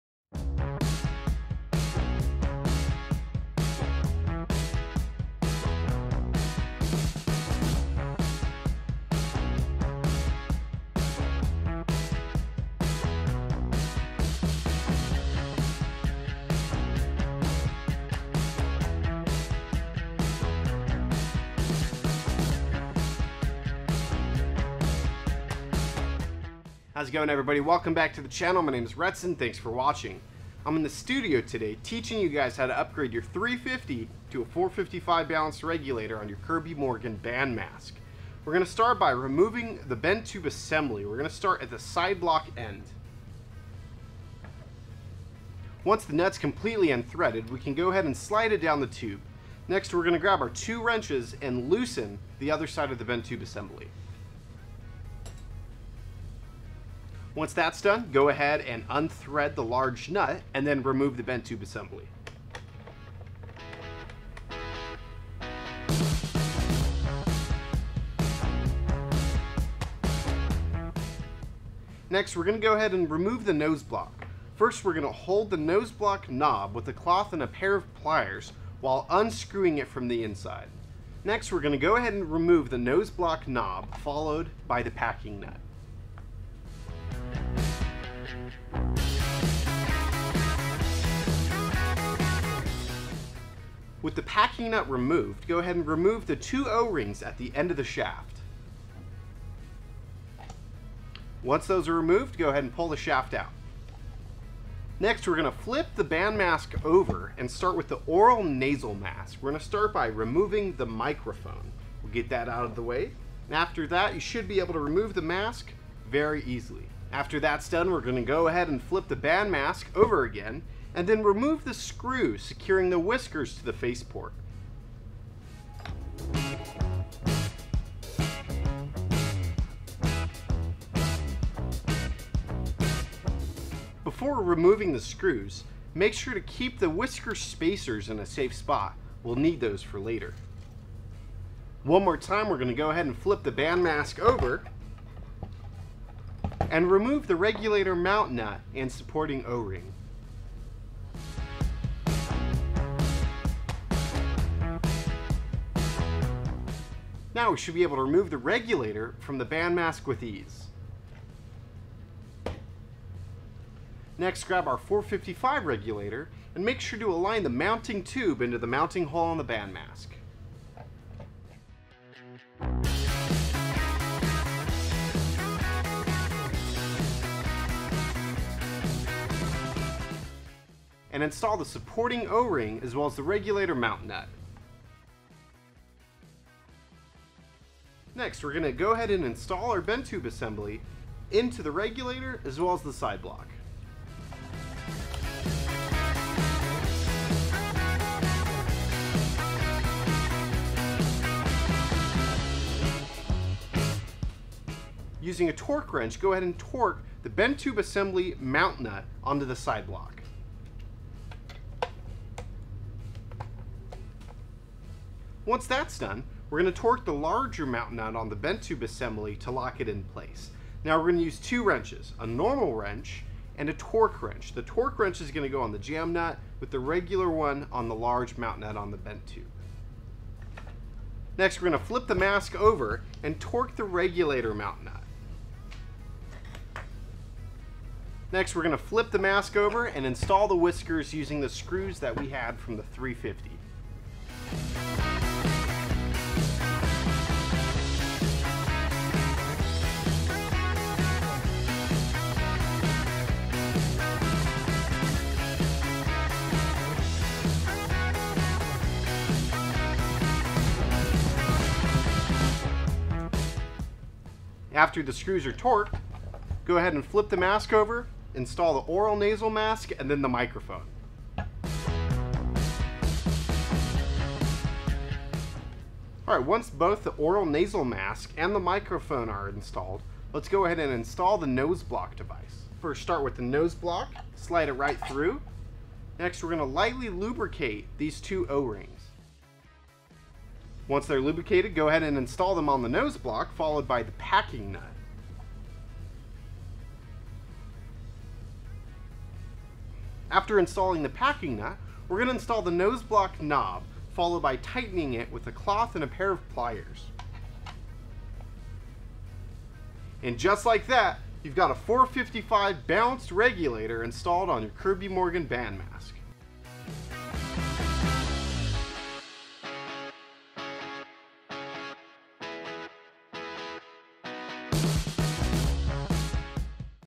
How To: Convert KM BandMask with SuperFlow® 350 to 455 Regulator
Every effort is made to ensure that the 455 balanced regulators, and other KMDSI regulators, are interchangeable among the wide variety of helmets and masks we produce. Please be aware that the final alignment of the regulator may vary from unit to unit, and this in no way will affect its function or performance.
The tools required to remove the SuperFlow® 350 regulator and install the 455 Balanced regulator into the helmet shell are the same.